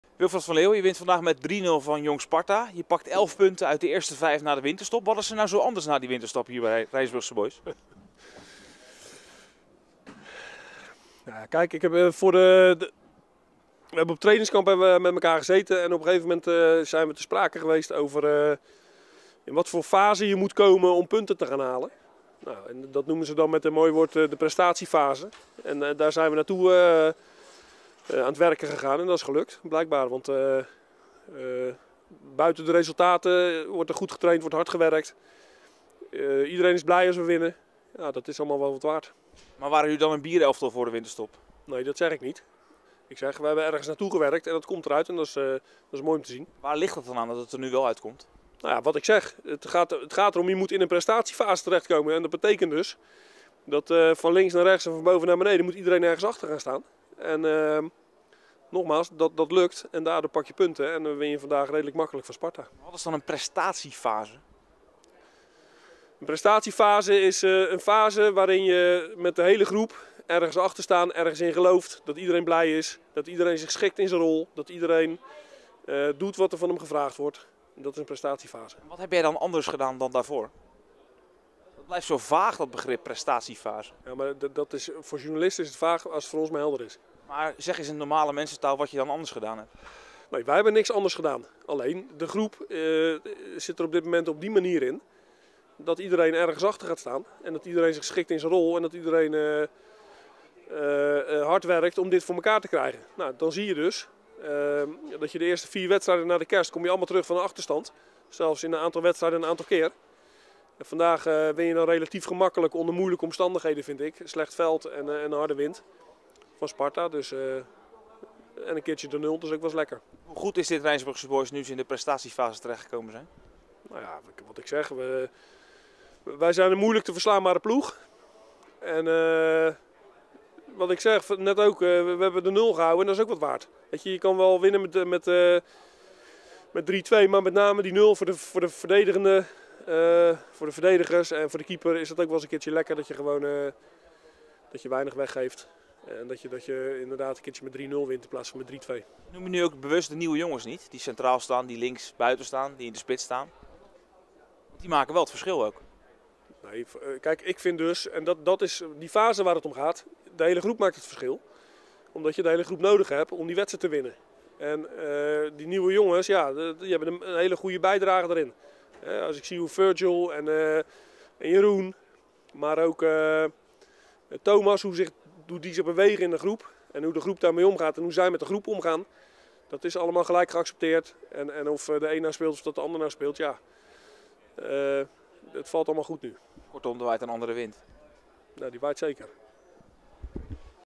Rijnsburg zit in 'prestatiefase': 'Voor journalisten vaag, voor ons helder' - OMROEP WEST SPORT
Rijnsburgse Boys had op de eigen Middelmors een makkelijk middagje tegen de talenten van Sparta. Na een 1-0 ruststand door een treffer van Jeroen Meefout was het de tweede helft aan Joel Tillema. De veelscorende speler zorgde in de 49e minuut dat de punten in de tas waren voor 'de Uien'. Furhgill Zeldenrust zette uiteindelijk de 3-0 eindstand op het scorebord.